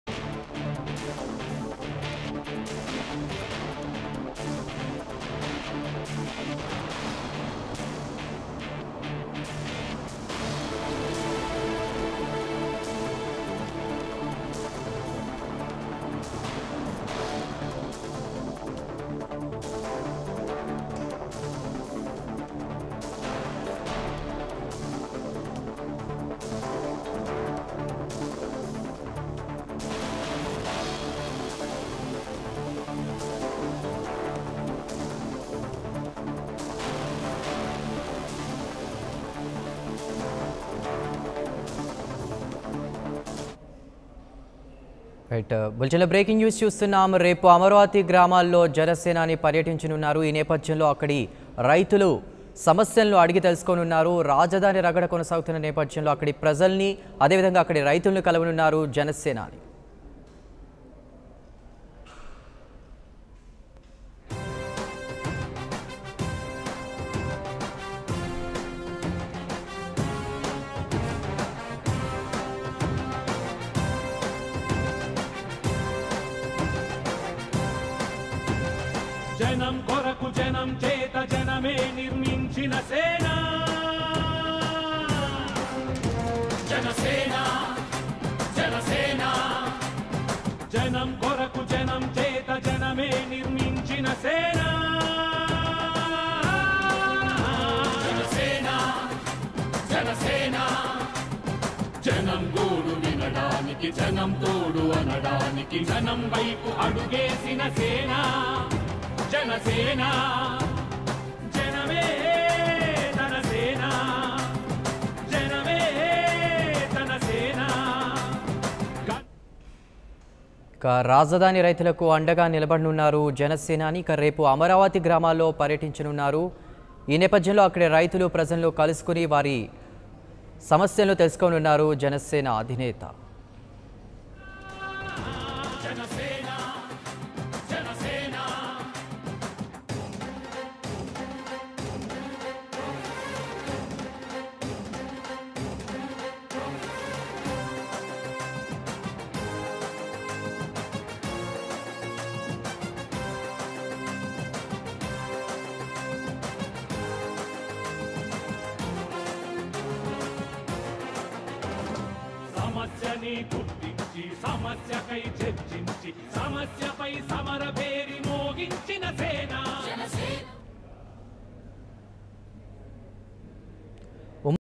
Janasena Chief Pawan Kalyan To Visit Amaravati Villages Tomorrow | 99TV Telugu
Janasena Chief Pawan Kalyan To Visit Amaravati Villages Tomorrow.